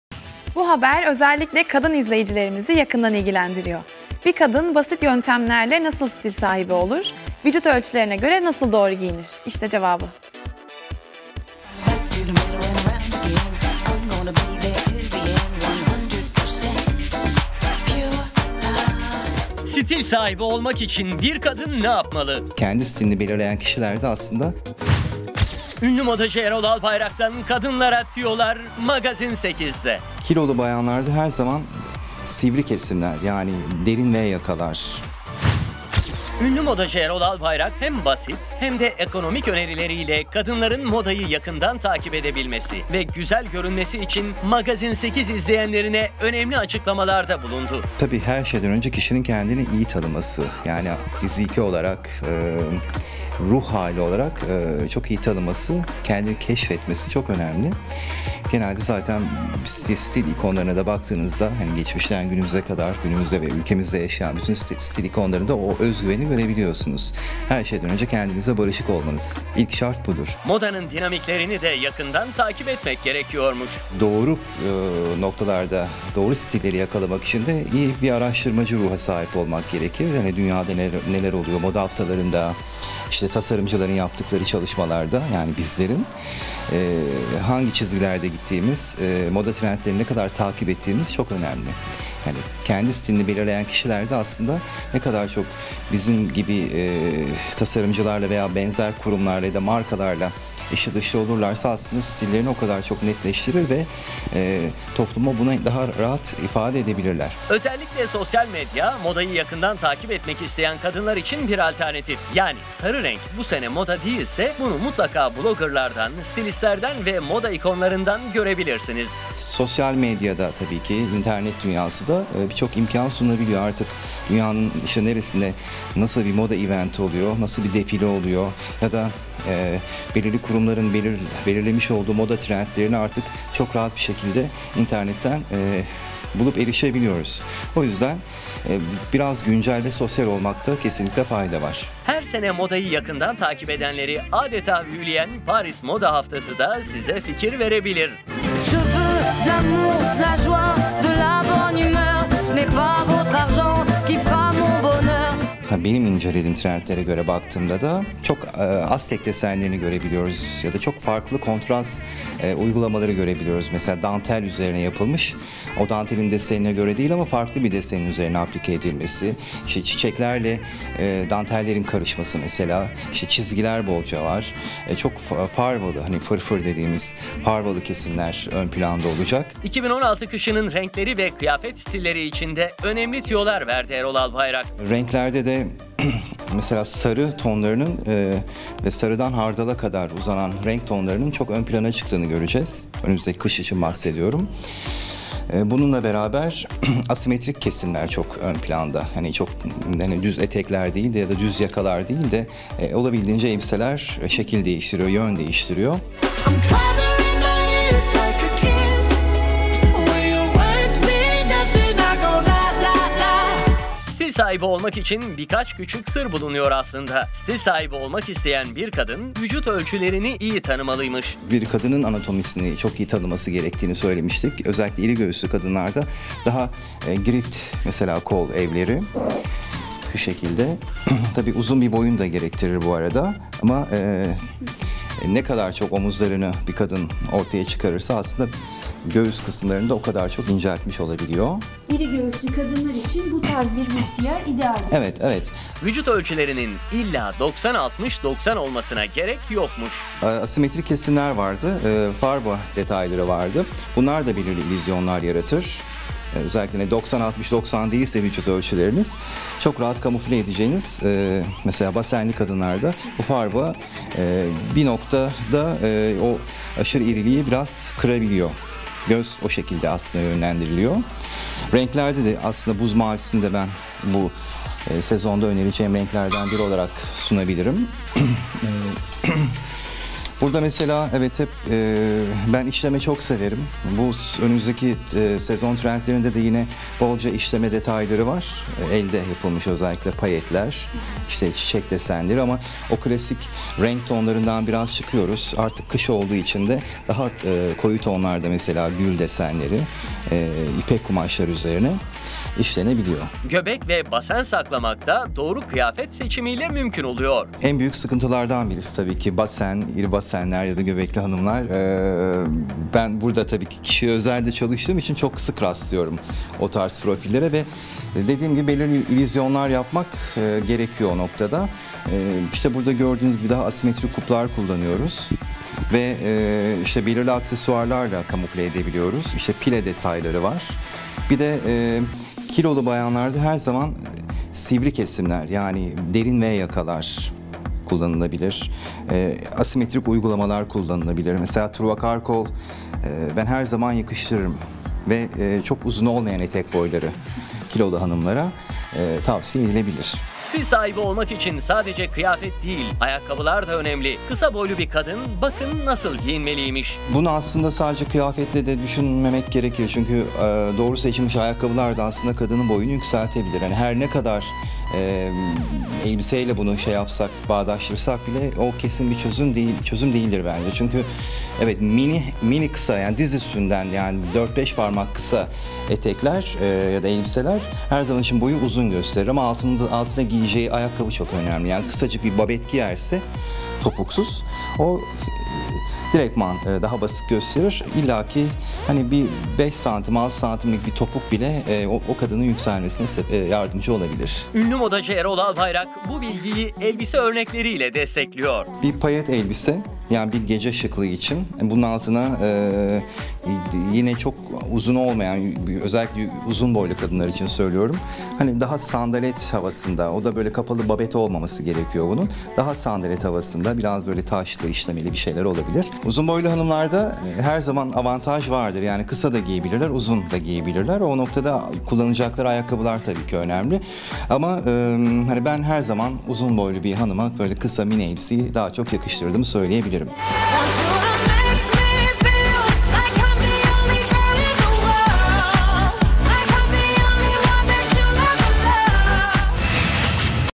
Kadınlar Stil Sahibi Olmak İçin Ne Yapmalı Ünlü Modacı Erol Albayrak Açıkladı
Kadınlar stil sahibi olmak için ne yapmalı ünlü modacı Erol Albayrak açıkladı.